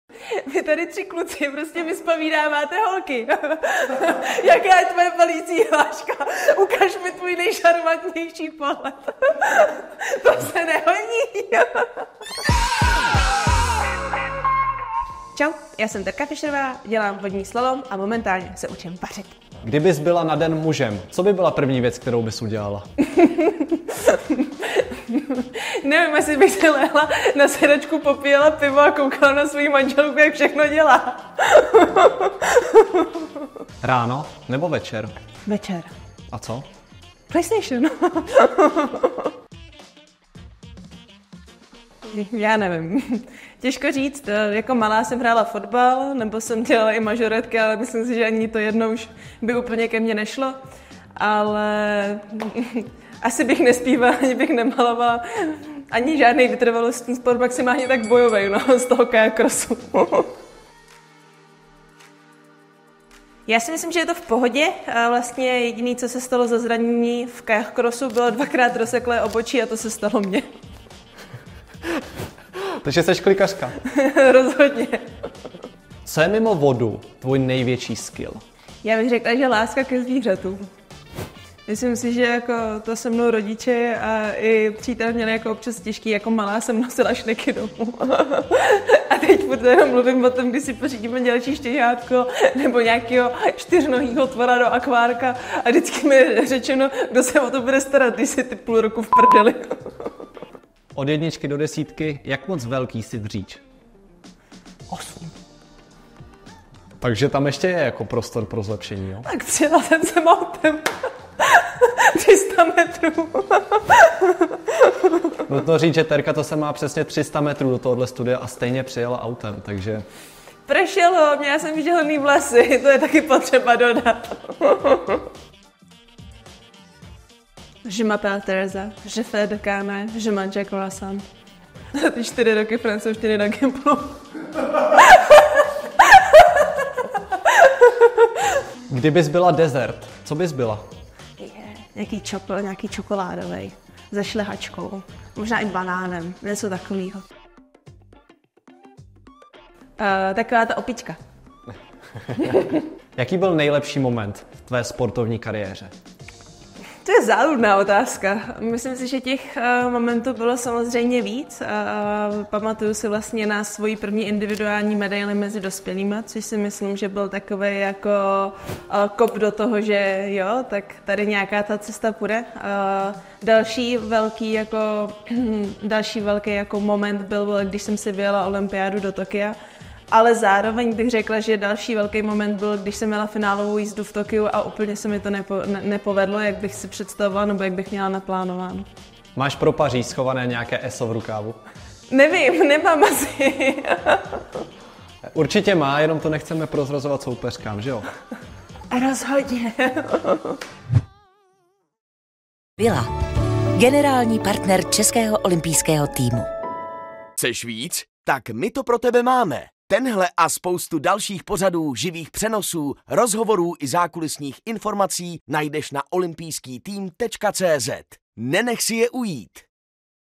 Kayak cross? V pohodě, až na pár rozseklých obočí | TEREZA FIŠEROVÁ
Které sporty dělala, než začala s divokou vodou? Jakou má bilanci zranění? A co pokládá za největší momenty své kariéry? Seznamte se s Terezou Fišerovou, která v Paříži reprezentovala Česko v nové olympijské disciplíně – kayak crossu.